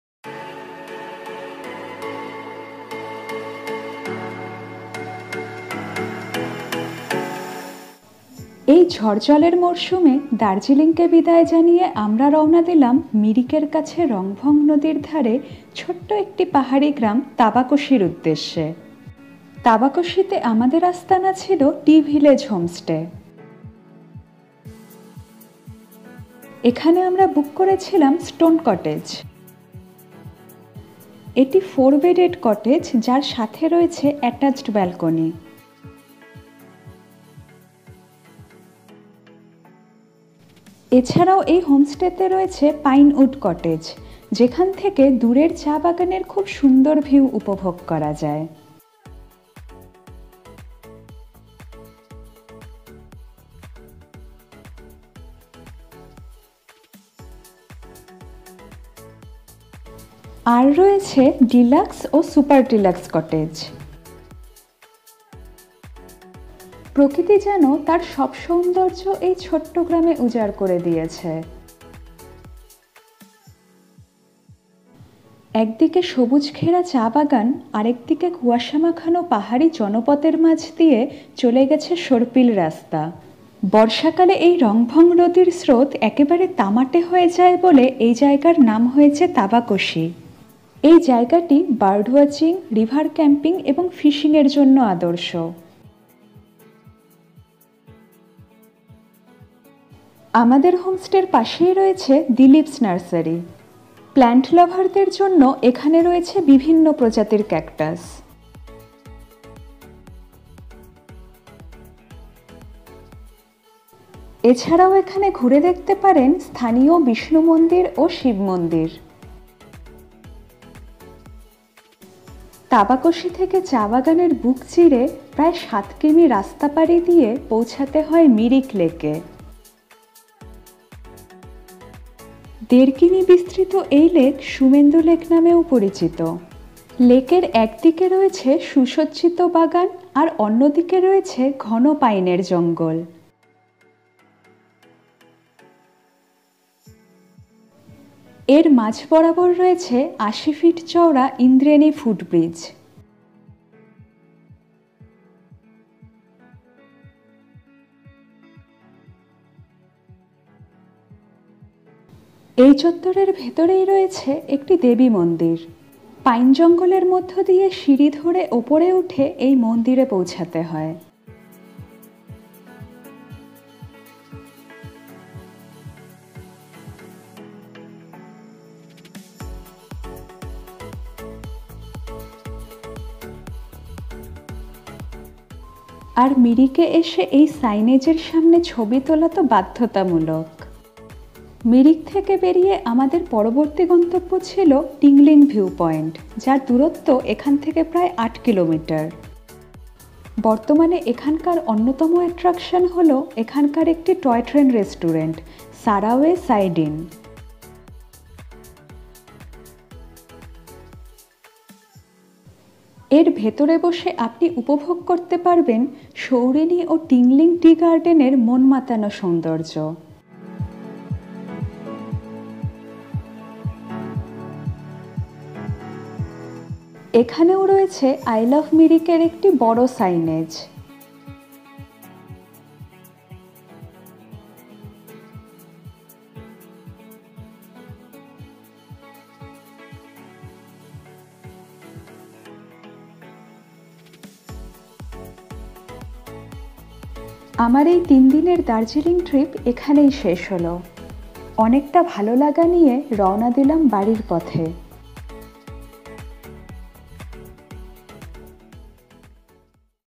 Tabakoshi | Rangbhang | Tea Village of Darjeeling | Mirik Lake | India's first Toy Train Restaurant
Room tariff for stone cottage: 1500/- per head

মিরিকের কাছে রংভং নদীর তীরে ছোট একটি পাহাড়ি গ্রাম তাবাকোশী। বর্ষাকালে এই নদীর স্রোত একেবারে তামাটে হয়ে যায় বলে এই জায়গার নাম হয়েছে তাবাকোশী। এখানে আমরা বুক করেছিলাম Tea Village Homestay এর Stone cottage। পাথরে তৈরী এই কটেজের সাথে রয়েছে attached balcony ও। এছাড়াও এই homestay তে রয়েছে Pine Wood Cottage, Deluxe cottage ও সুপার deluxe cottage।

একদিকে চা বাগান, আরেকদিকে কুয়াশা মাখানো পাহাড় জনপদের মাঝ বরাবর চলে গিয়েছে সর্পিল রাস্তা। এই গ্রামটি bird watching, river camping ও fishing এর জন্য একটি আদর্শ জায়গা।

এখন থেকে নিকটবর্তী টুরিস্ট স্পট হলো মিরিক লেক। লেকের একদিকে রয়েছে সুসজ্জিত বাগান, আরেকদিকে ঘন পাইনের জঙ্গল। আর জঙ্গলের ভেতরে রয়েছে একটি দেবী মন্দির।

এখান থেকে প্রায় 8km দূরে আরেকটি আকর্ষণীয় টুরিস্ট স্পট হলো Tingling View Point। বর্তমানে এখানকার অন্যতম attraction হলো একটি Toy Train Restaurant, যার ভেতরে বসে আপনি উপভোগ করতে পারবেন সৌরিনী ও টিংলিং চা বাগানের মনমাতানো সৌন্দর্য্য।